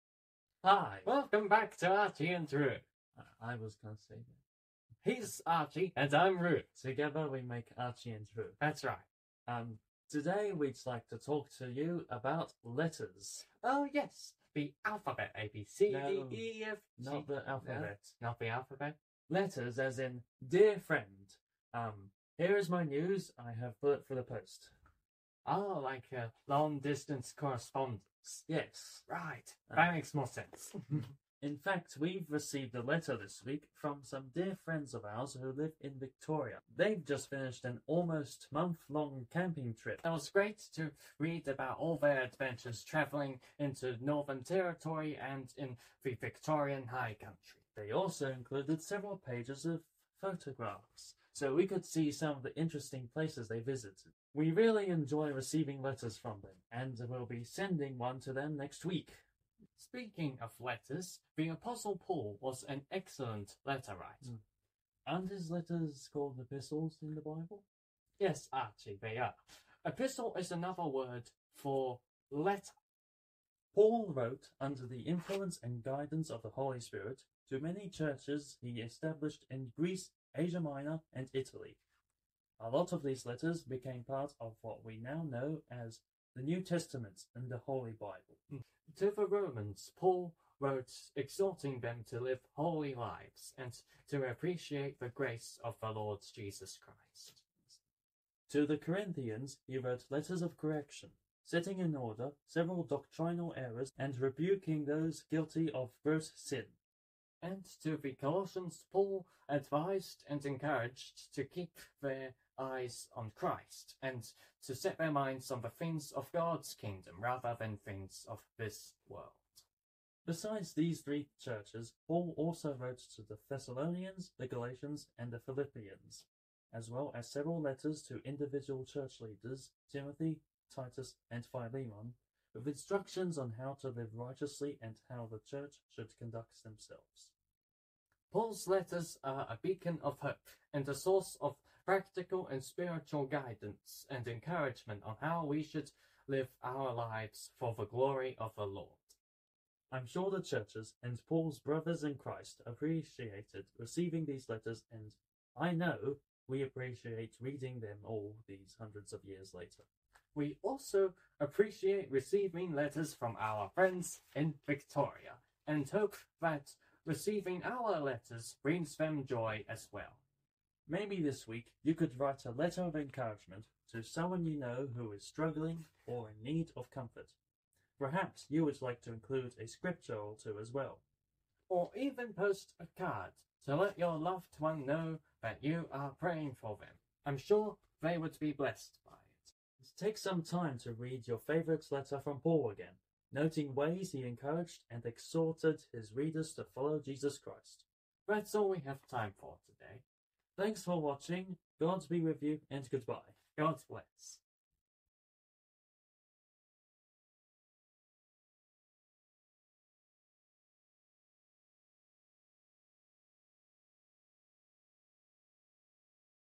Friends, letters, encouragement.
Archie and Roo hope to encourage you today in God's word and to be a friend to some one in need this week.